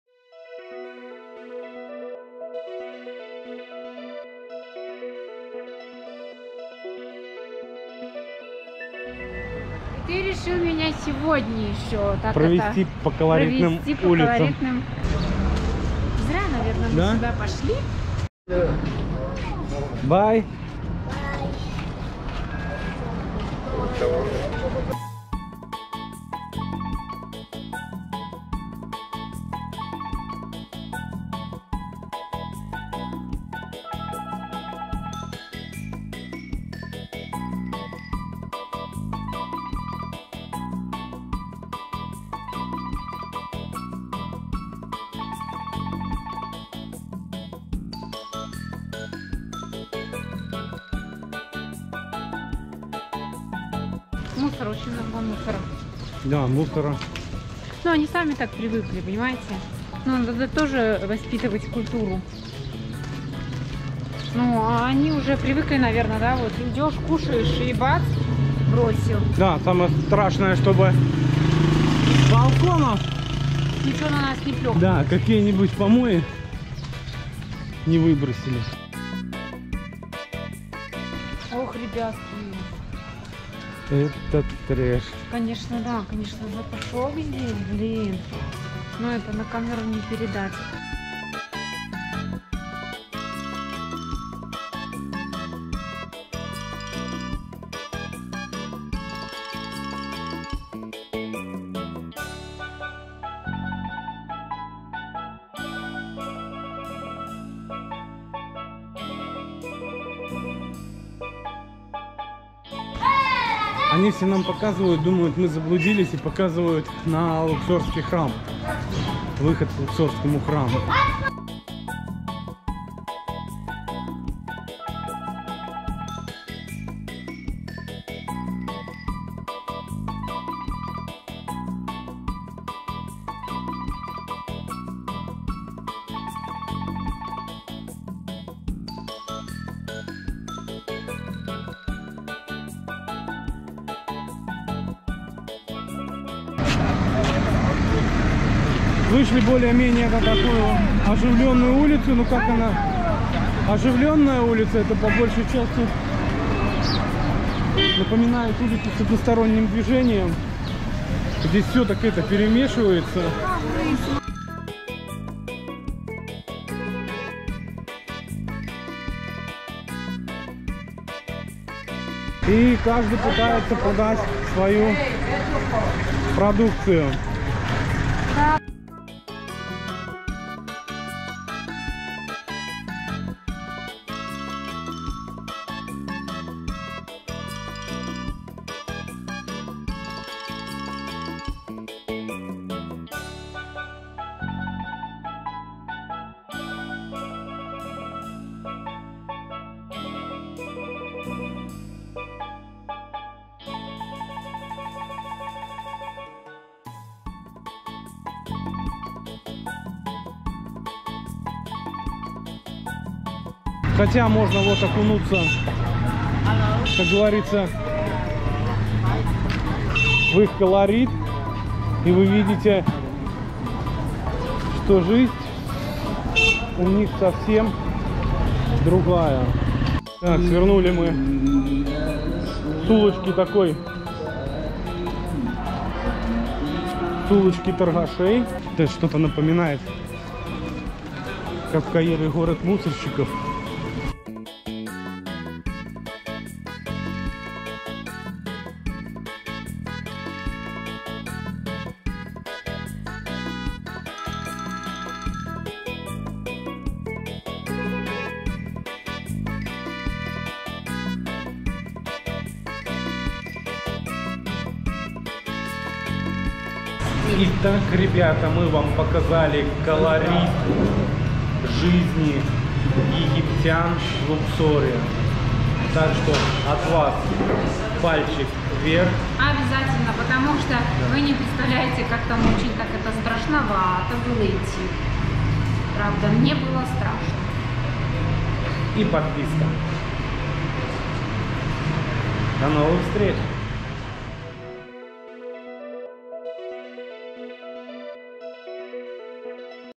Египет, которого не показывают туристам.
4К. Шокирующие улицы Луксора.
Трущобы Египта.
Реальный Египет.
На улицах Луксора можно купить любой товар.
Помощь на покупку камеры    Help with buying a camera